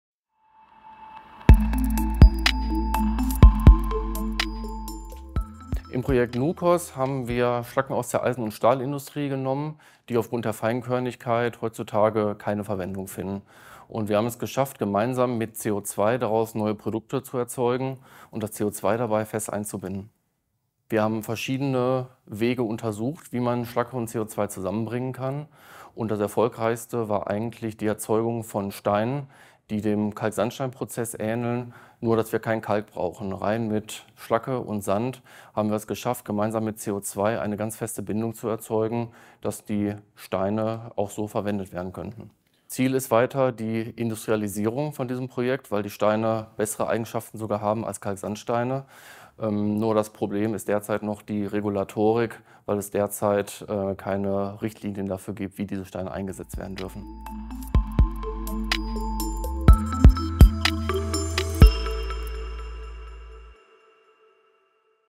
Kalksandstein 2.0 - Entwicklungen im Projekt NuKoS (CO2-WIN BMBF)
"NuKoS – Nutzung von Kohlenstoffdioxid in Schlacken aus Stahl- und Metallproduktion"

Im Projekt "NuKoS" wurden Schlacken, welche heutzutage als Abfall gelten, mit CO2 so umgesetzt, dass Steine erzeugt werden konnten, welche als Ersatz für herkömliche Kalksandsteine genutzt werden könnten. Die CO2-basierten Steine haben bessere Eigenschaften und kommen ohne die Nutzung von Kalk aus. Wenn der regulatorische Rahmen gesetzt sein wird, können die Steine vermarktet werden.

Hier erfahren Sie mehr über die Fördermaßnahme "CO2 als nachhaltige Kohlenstoffquelle – Wege zur industriellen Nutzung" (CO2-WIN) des Bundesministeriums für Bildung und Forschung